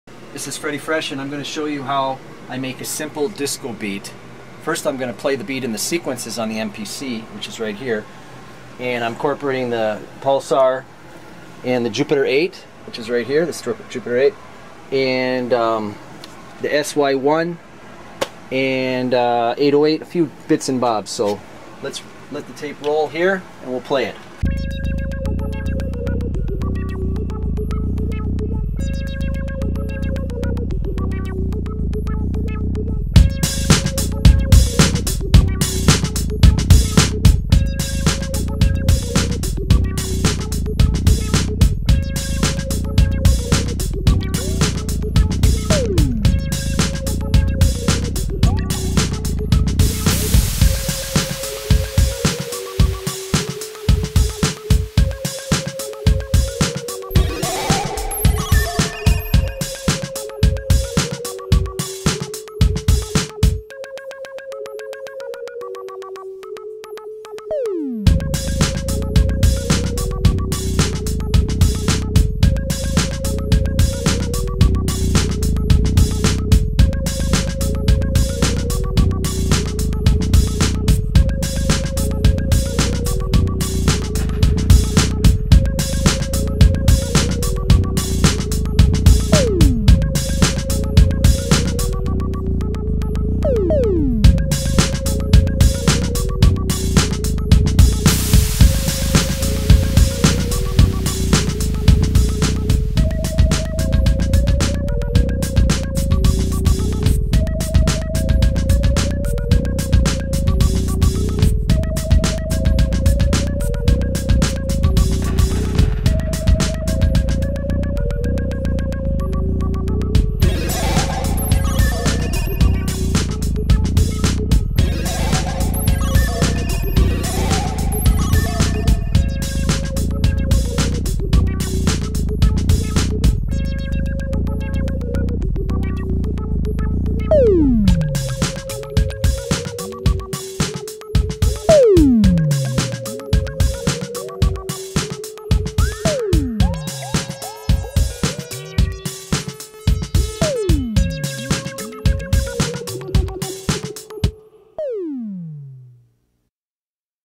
Slow Disco Beat with Jupiter 8 Arpeg. and Pulsar Drum and Bass
Quick MPC 4000 run thru with Pulsar and Jupiter 8, SY1, Norand Mono